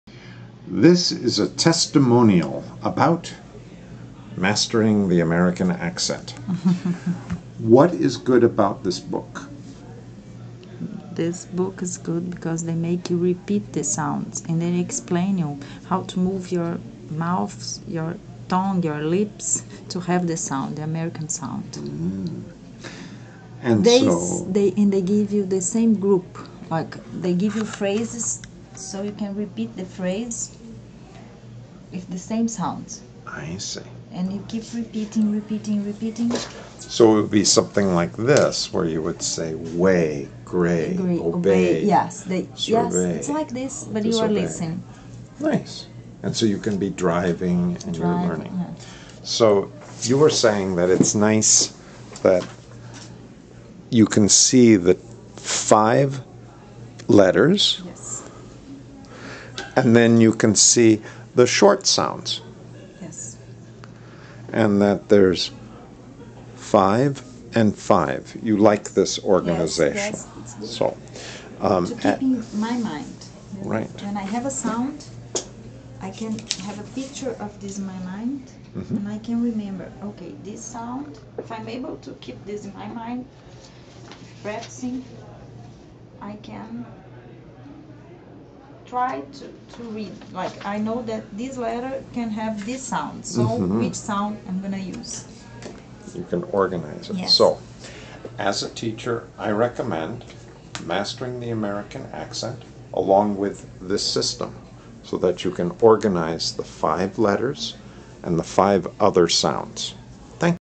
1 of 4 Havana Pronunciation System You can ask for the FREE IT SOUNDS LIKE worksheet
Testimonial about Mastering the Americna Accent book and audio CD

plus the IT SOUNDS LIKE method with 14 boxes

write to FREEenglishLESSONS  a t     g   m a  i  l       D/O/T   c  o  m

The testimonial is by Rita

we hope you will share this information

The University of Havana Pronunciation System does not use special symbols to indicate the sound.   The International system called IPA uses "backwards C" and "Upside down V"

This series of short videos is inspired by the FULLY GUIDED INSTRUCTION approach recommended by Dr, Richard Clark.

Please send comments to Steve at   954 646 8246

Camera work by Mario LLorente